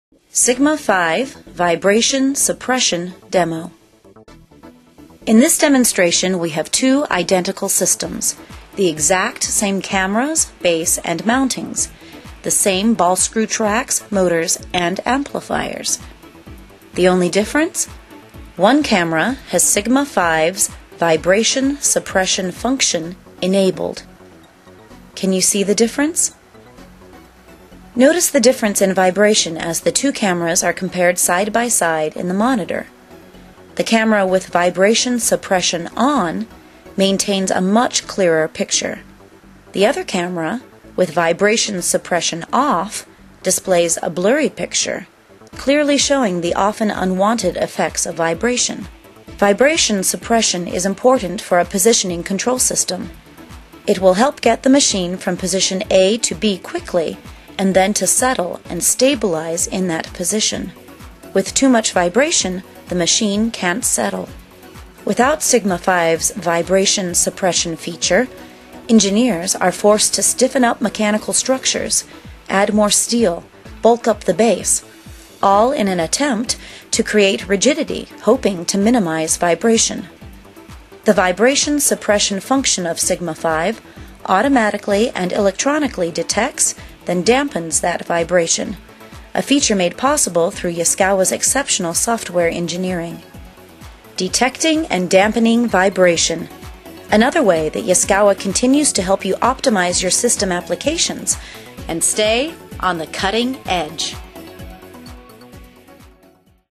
Sigma-5- Vibration Demo
This video showcases the superior vibration suppression features of the Sigma-5.